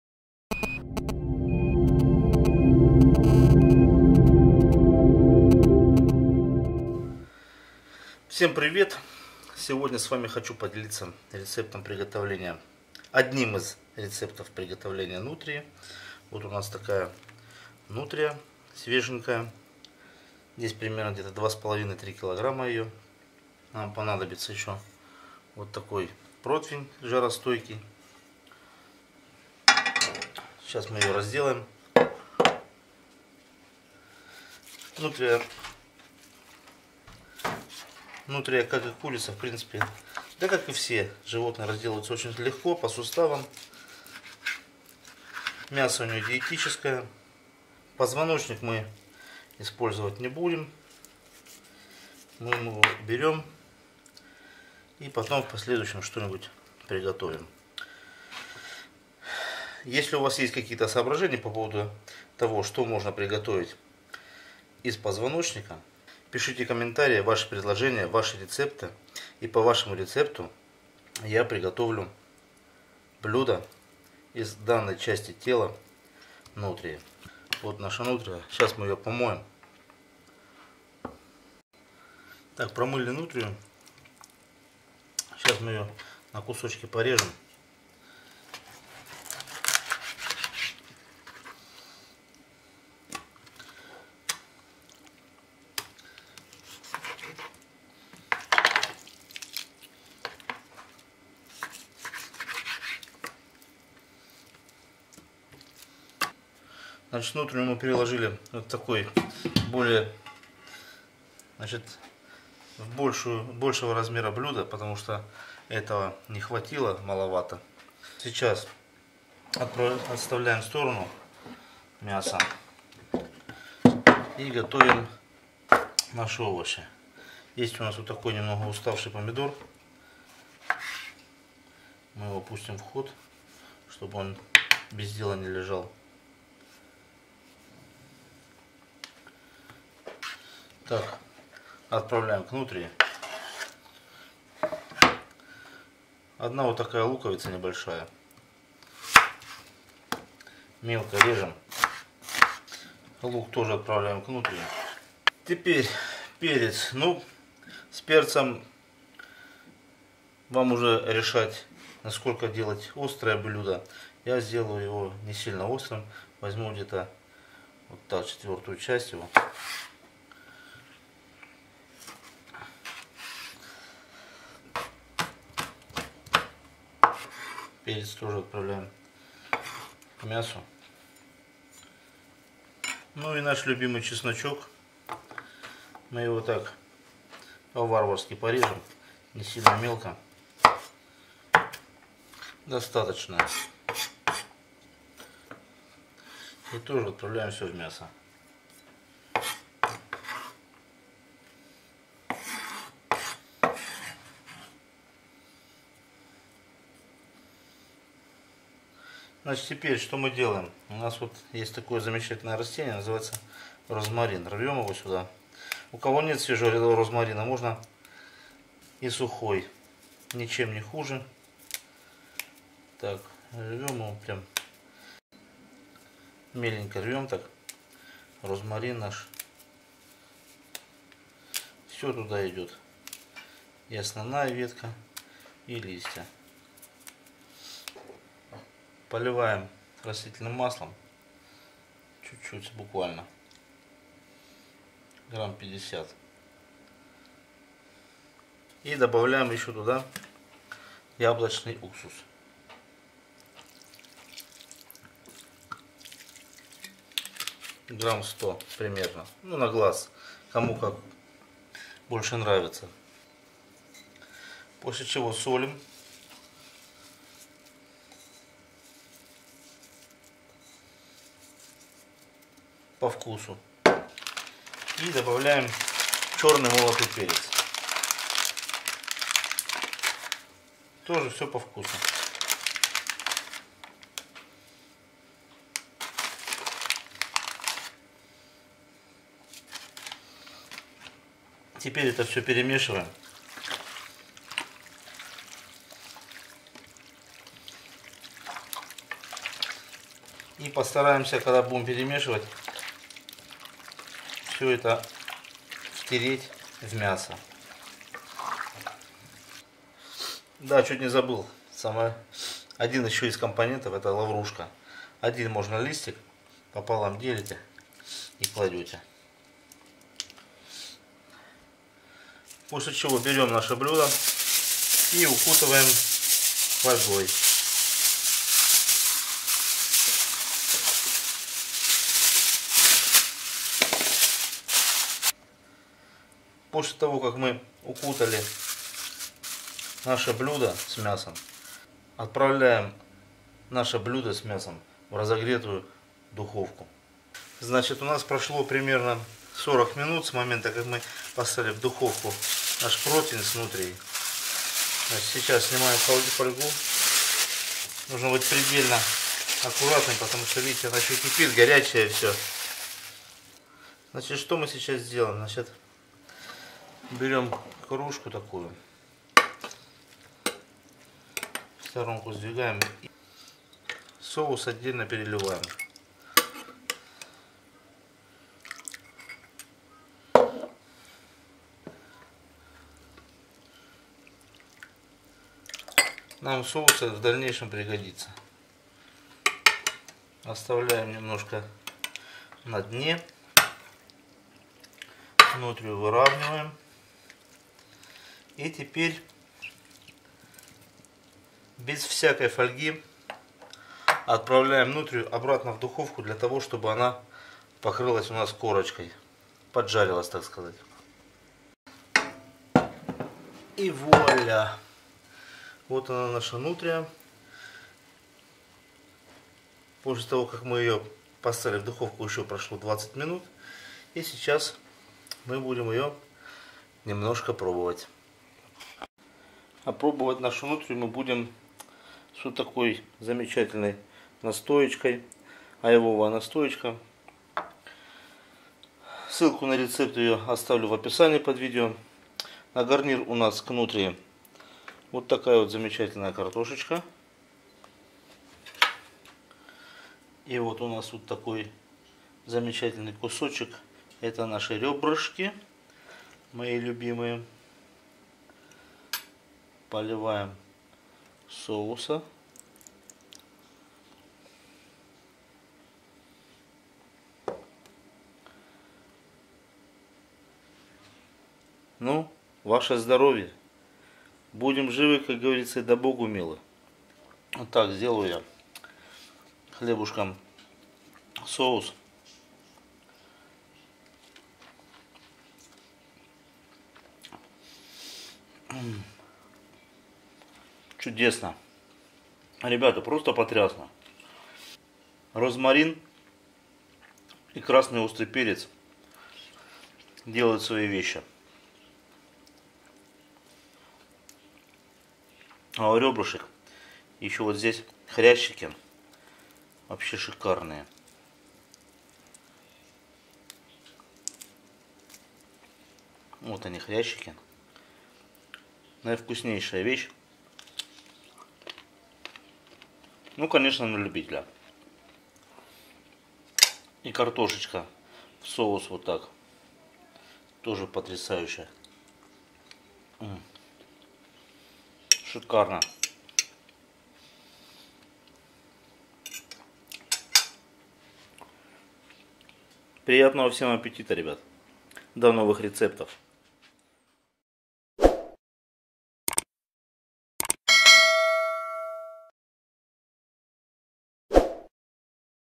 Рецепт приготовления Нутрии Простой и Вкусный рецепт
Рецепт приготовления нутрии простой и вкусный рецепт.Требуется минимум затрат, в результате, нутрия, получается очень вкусной и сочной.
Мясо нутрии - 1 кг.
соль,перец, пряности по-вкусу.